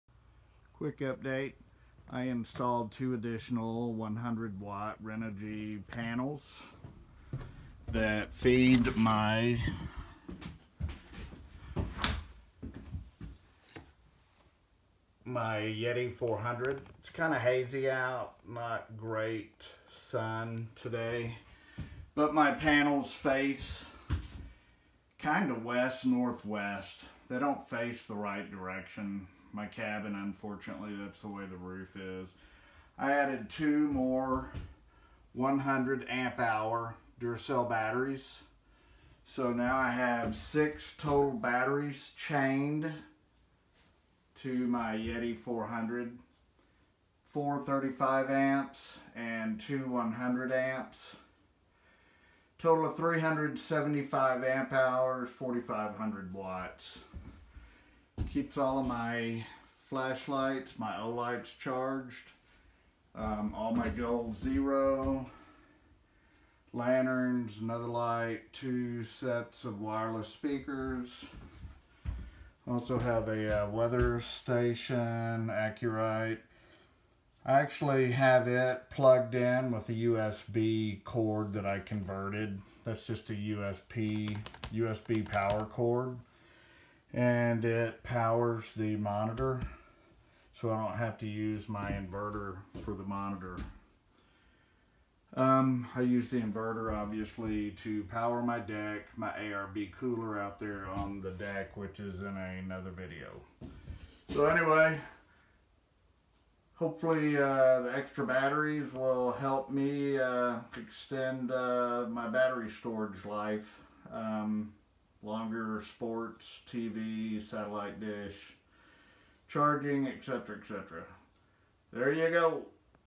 Yeti 400 Chain Upgrade - Chaining - Review
Installed 2 additional 100 amp/hr batteries...